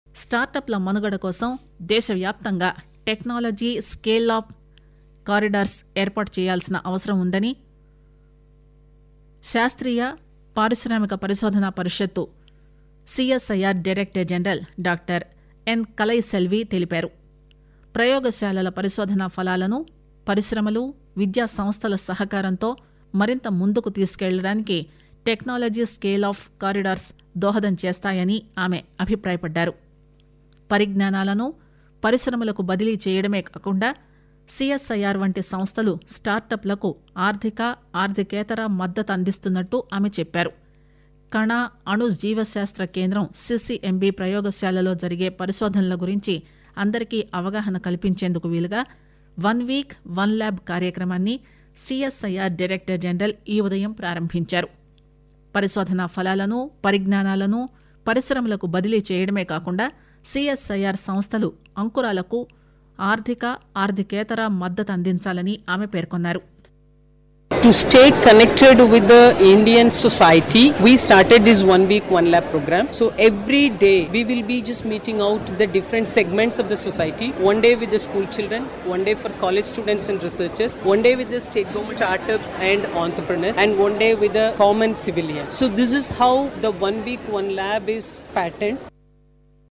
'టెక్నాలజీ స్కేల్ అప్ కారిడార్స్' - Dr N Kalaiselvi inaugurates first "One week- One lab"
Director General of Council of Scientific and Industrial Research (CSIR)
 Dr N Kalaiselvi inaugurates first "One week- One lab" initiative at Centre for  Cellular and Molecular Biology (CCMB).